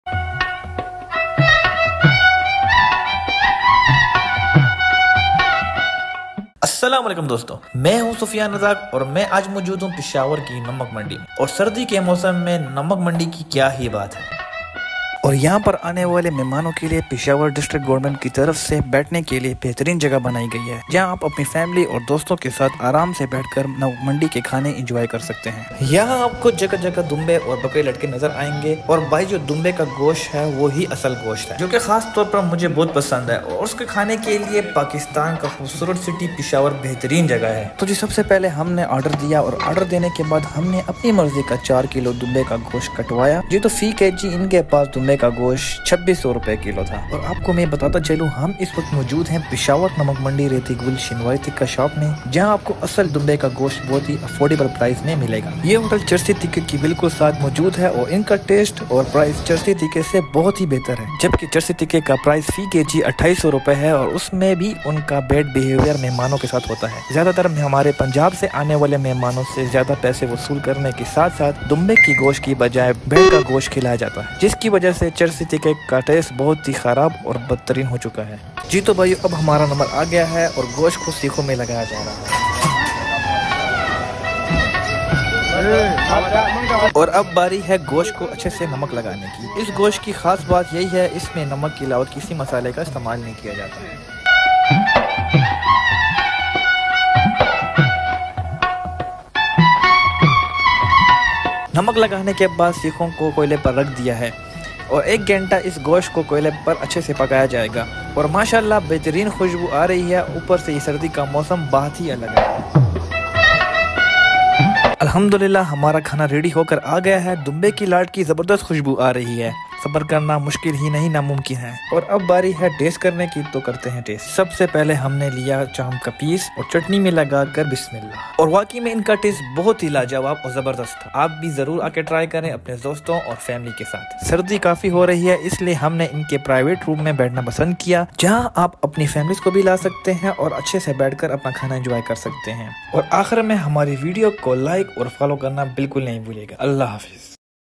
Redi Gul Shinwari Tikka Shop | Namak Mandi Peshawar | Food Street Peshawar
Explore the culinary wonders of Peshawar. Immersing you in the vibrant atmosphere of the renowned Namak Mandi! Join me as I venture into the iconic Redi Gul Shinwari Tikka, a legendary eatery celebrated of its delectable Namkeen Tikka.

📍Namak Mandi Rd, Shah Qabool Colony Namak Mandi, Peshawar, Khyber Pakhtunkhwa 25000, Pakistan